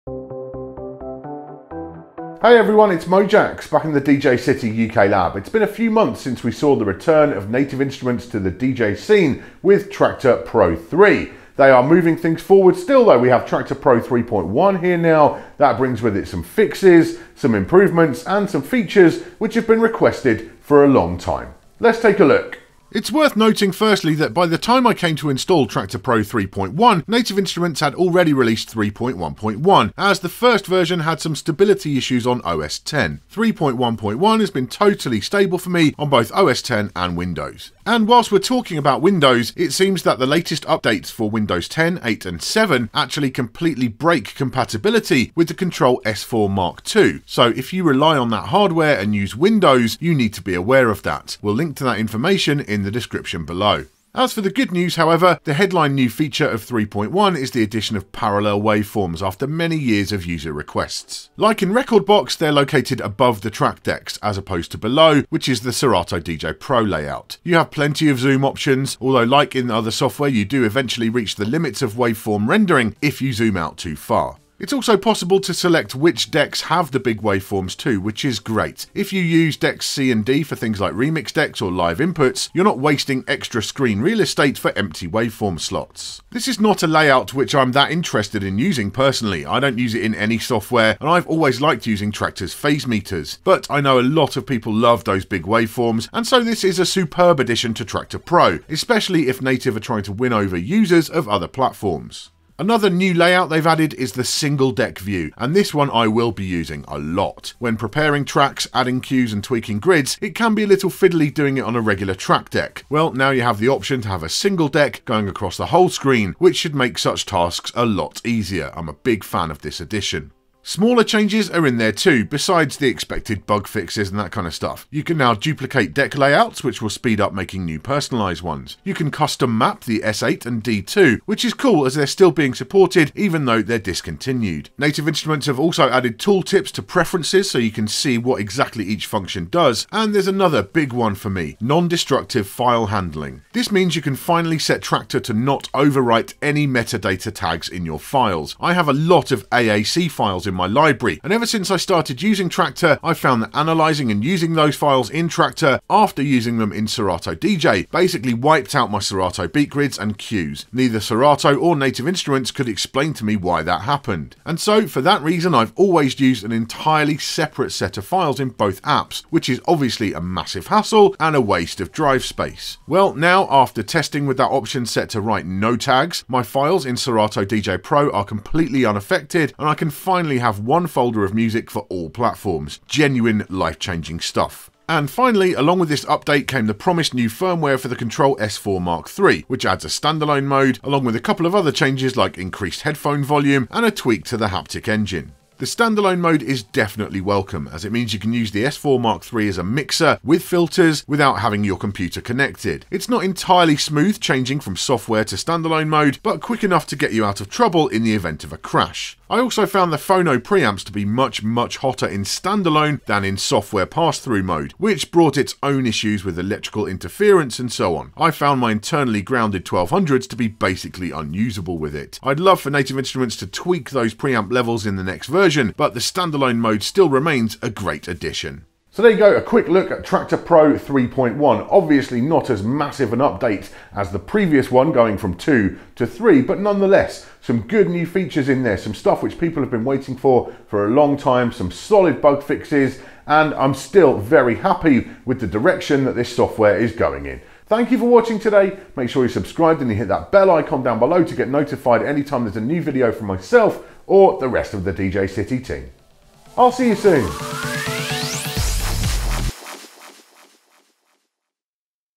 First Look: Native Instruments Traktor Pro 3.1 | Tips and Tricks
Mojaxx takes a first look at Native Instruments' Traktor Pro 3.1, which includes bug fixes, tweaks, and for the first time, parallel waveforms.